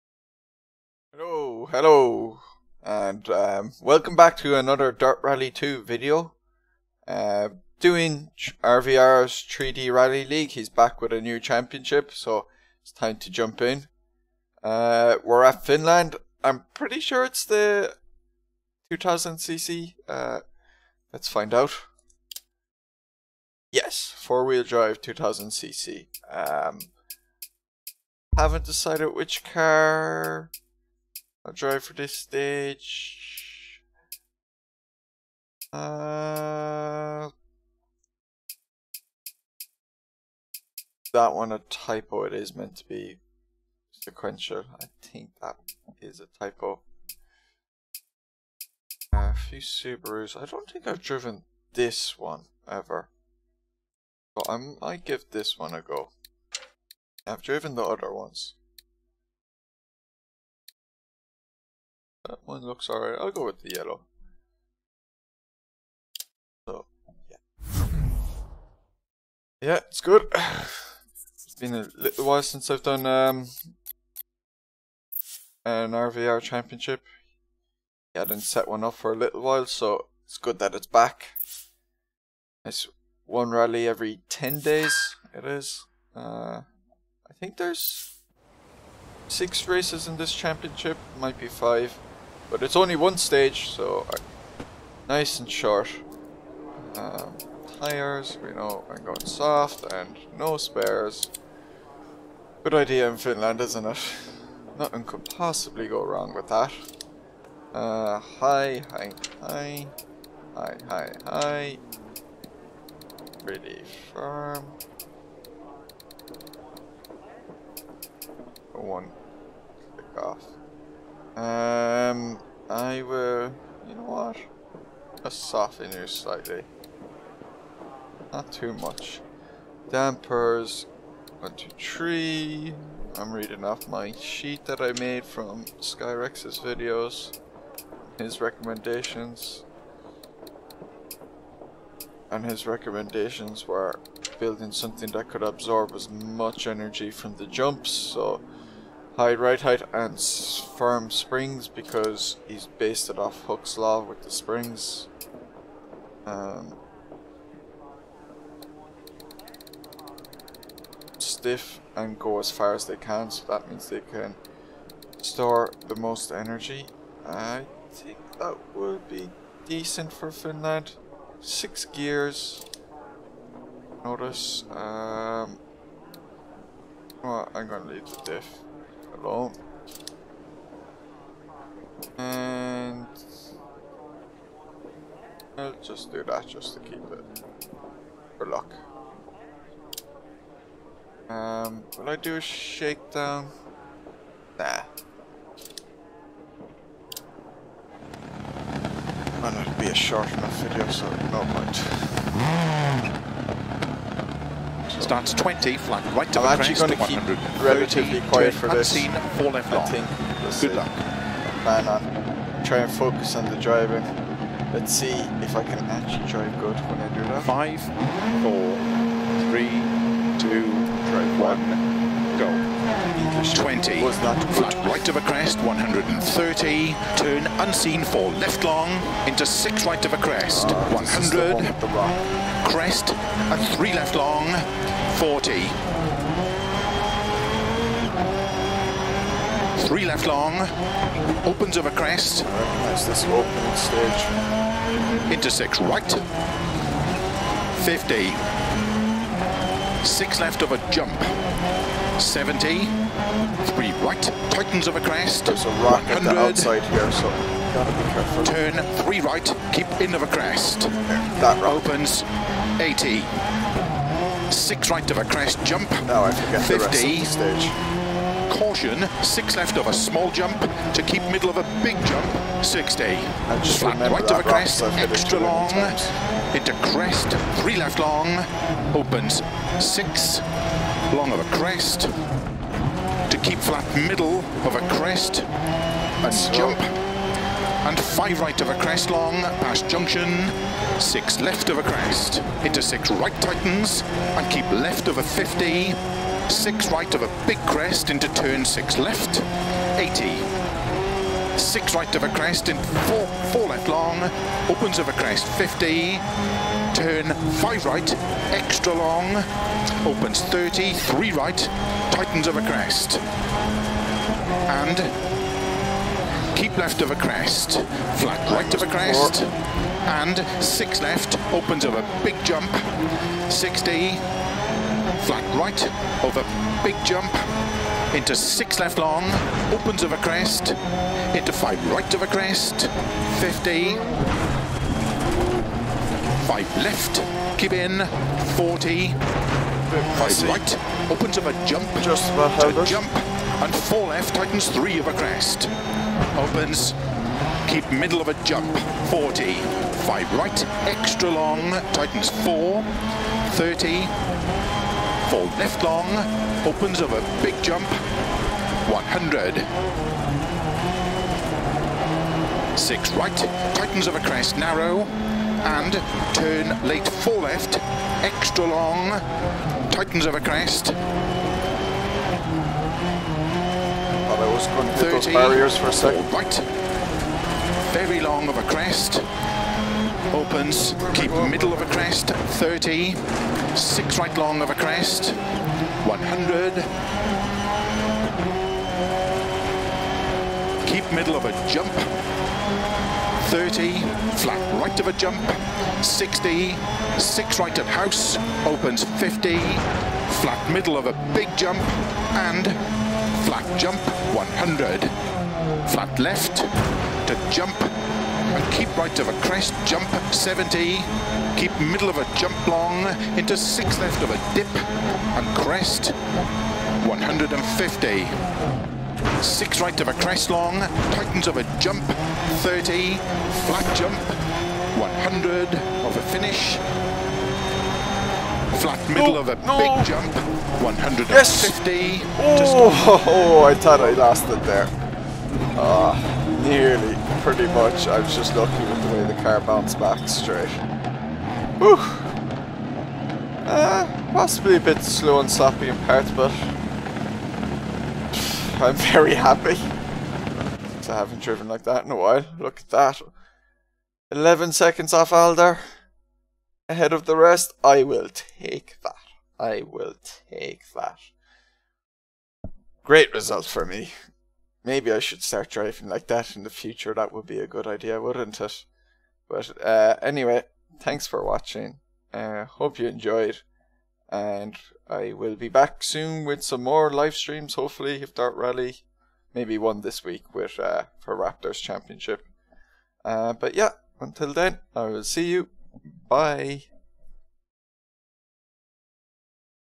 Rally: 3D Rally League, 2000cc 4WD Championship, Event 1, Finland.
After some time off, R-VR Closet is back hosting a new championship for the 3D Rally League. These rally championships are composed of 6 rounds with just one stage in each round, meaning that it is a short frantic sprint when we drive. This time we are doing a championship that uses the 2000cc 4WD cars. The first round is at Finland.

I have got sick of my recent bad driving in my recent rallys, so instead of talking too much I decided before hand to focus more on my driving for this one. Will I succeed? Or will I forget about this aim, talk over a call for a turn, miss it and crash straight into the tree?